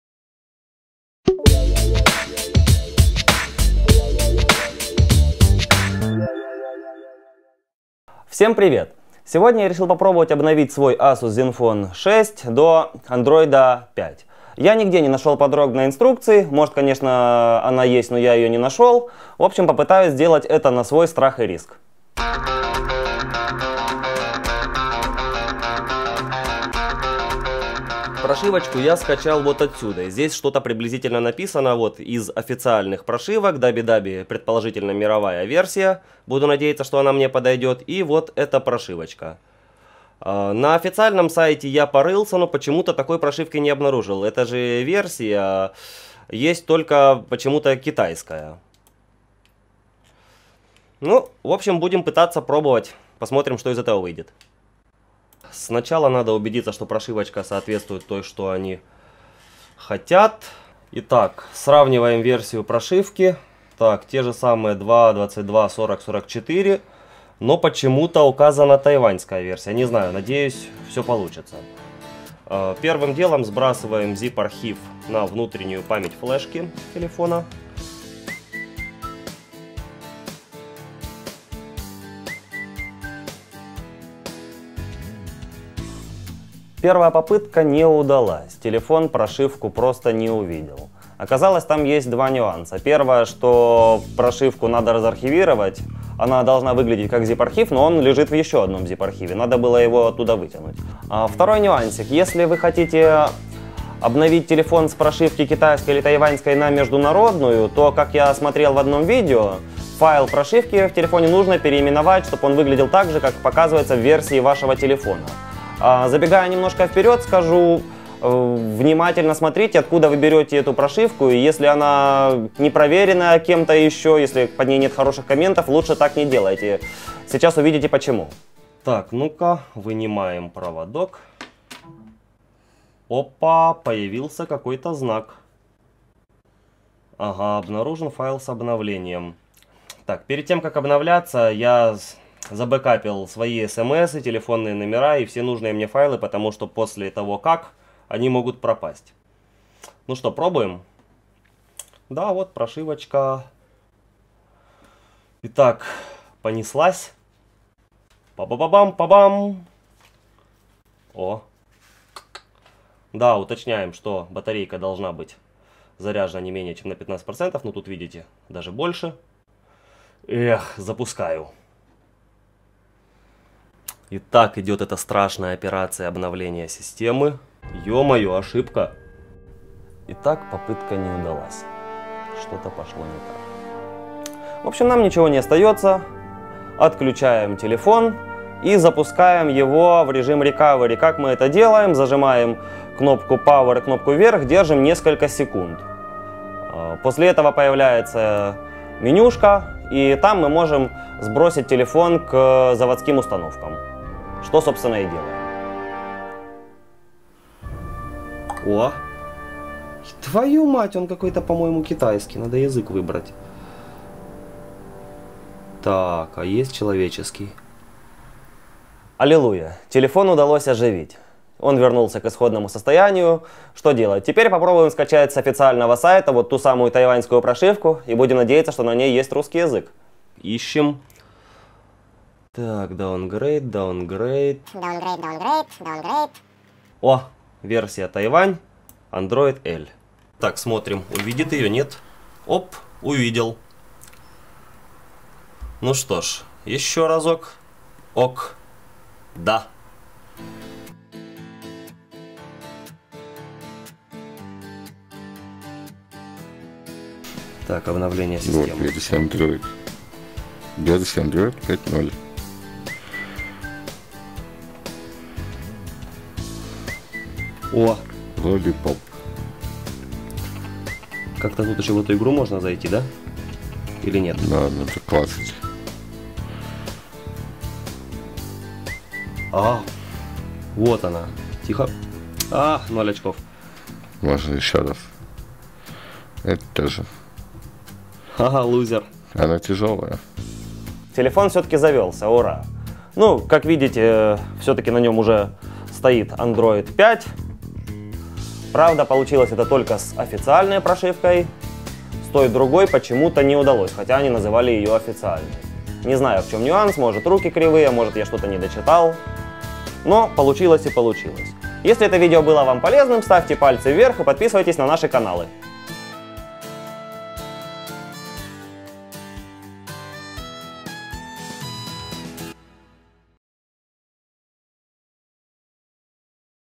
Как обновить прошивку ASUS ZenFone 6 на Android?
Обновление прошивки ZenFone 6 с 4.4 KitKat До Android 5.0 Lollipop.